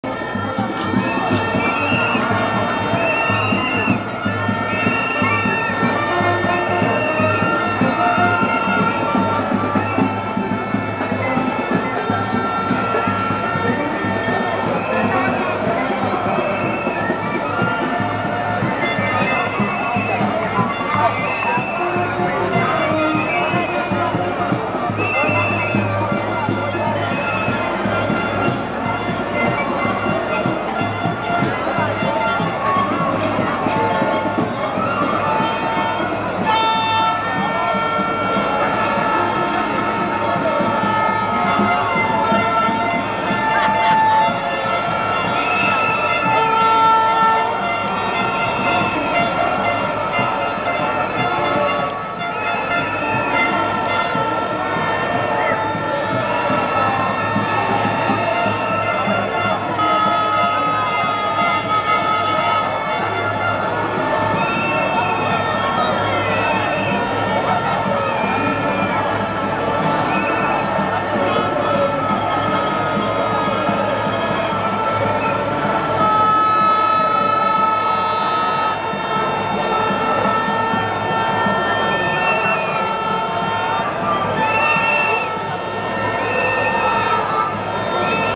Plaza de Bolivar
El primero de mayo de 2013 estuvimos en plaza de Bolívar